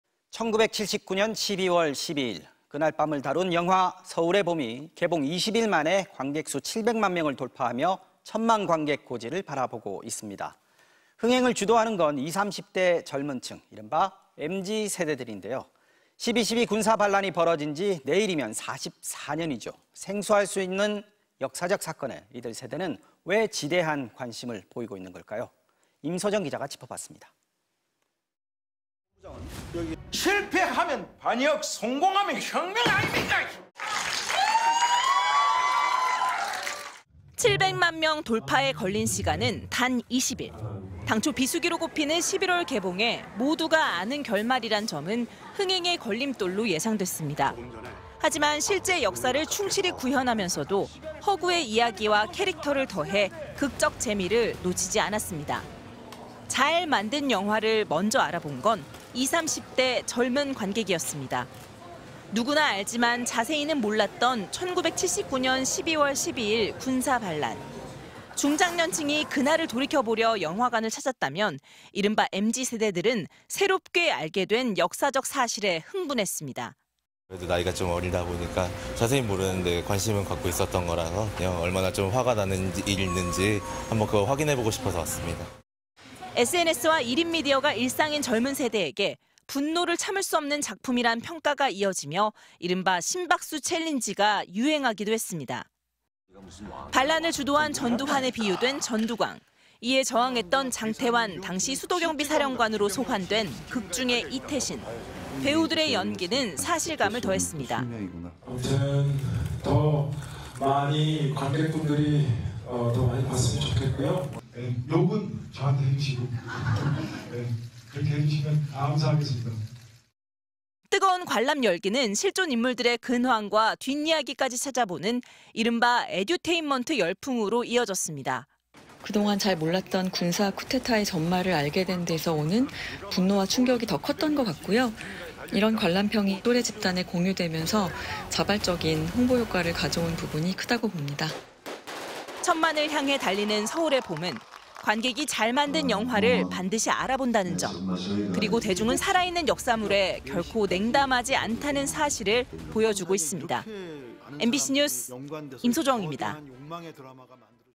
'1천만' 향하는 '서울의 봄', MZ열풍 이유는? (2023.12.11/뉴스데스크/MBC)
1979년 12월 12일, 그날 밤을 다룬 영화 '서울의 봄'이
개봉 20일 만에 관객수 7백만 명을 돌파하며
천만 관객 고지를 바라보고 있습니다.
흥행을 주도하는 건 2~30대 젊은 층, 이른바 MZ 세대들인데요.
12.12 군사반란이 벌어진 지 내일이면 44년이죠.
생소할 수도 있는 역사적 사건에 이들 세대는
왜 지대한 관심을 보이고 있는 걸까요?